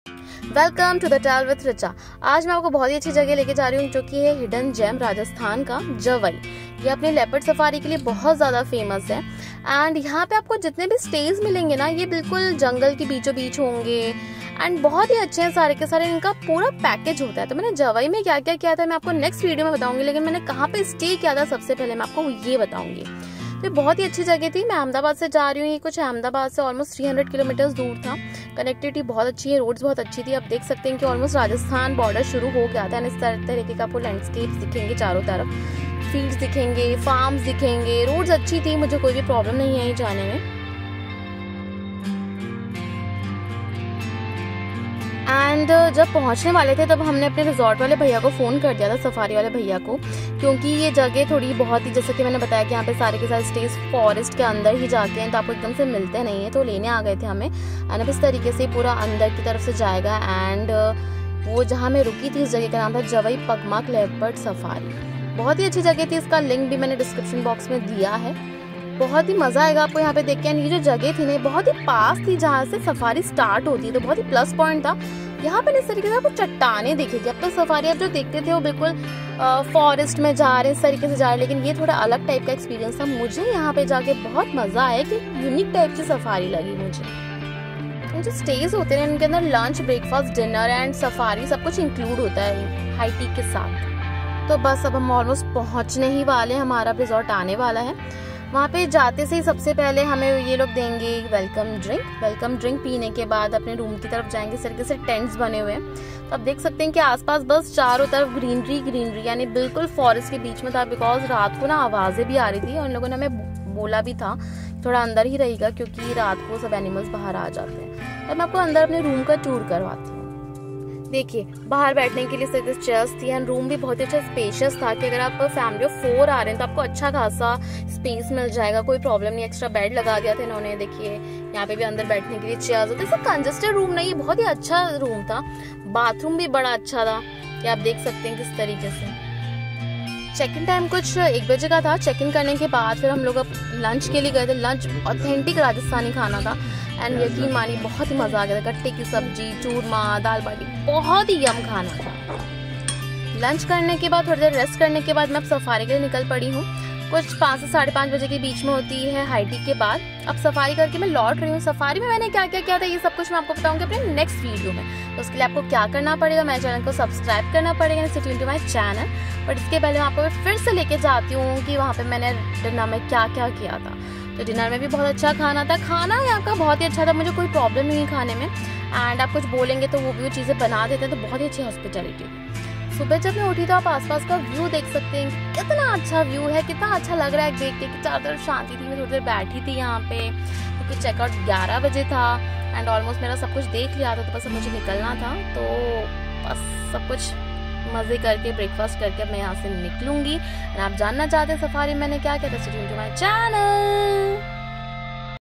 Jawai Leopard Safari // Jawai pugmark leopard safari
one of the hidden gem in India

I loved the place as it will give you different level of adventure